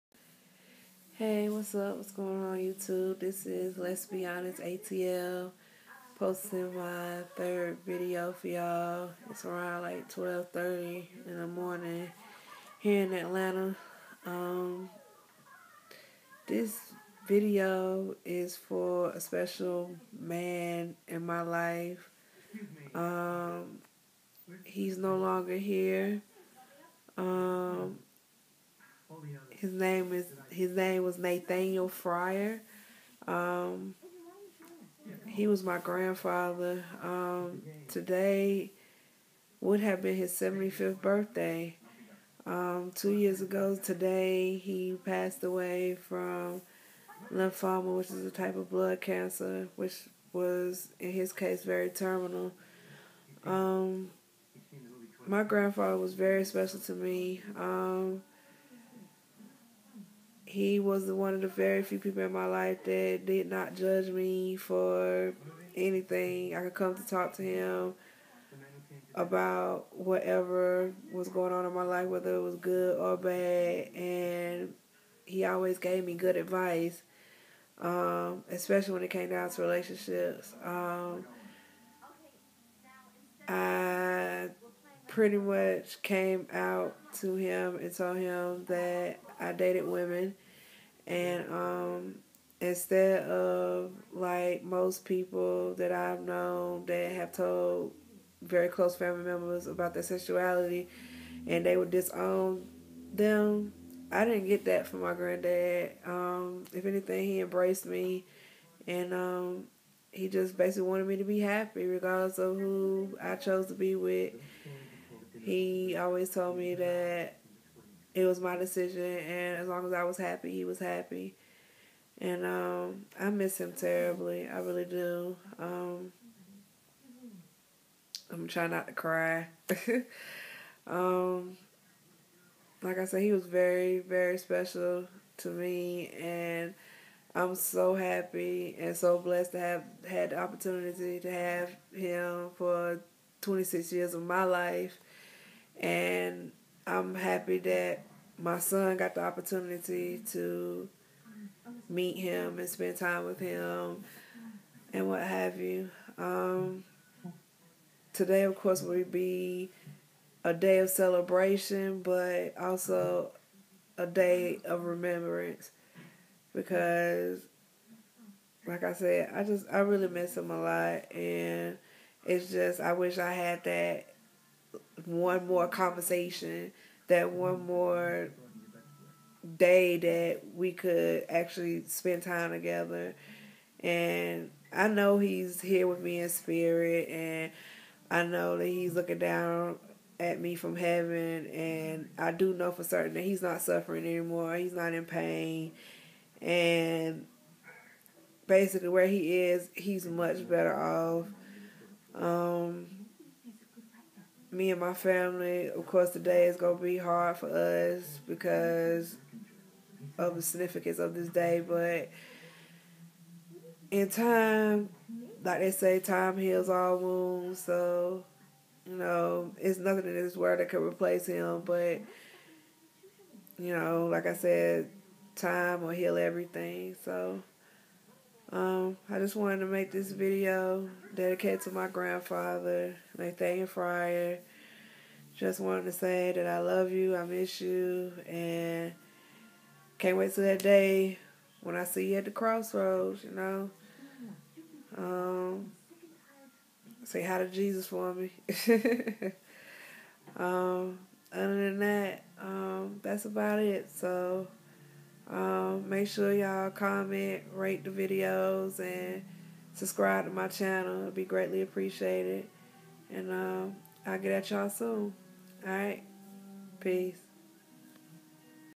R.I.P. 2 my Grandad...Nathaniel Fryar...Happy Birthday
2Day would have been his 75th birthday, he passed on his birthday 2 years ago